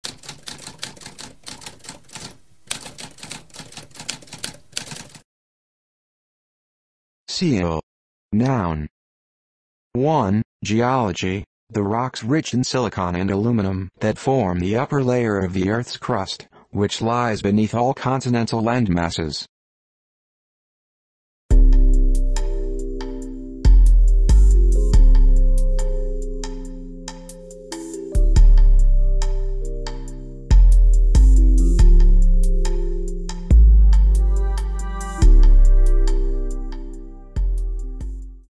What does sial mean?
A spoken definition of sial.

Intro Sound:
Typewriter - Tamskp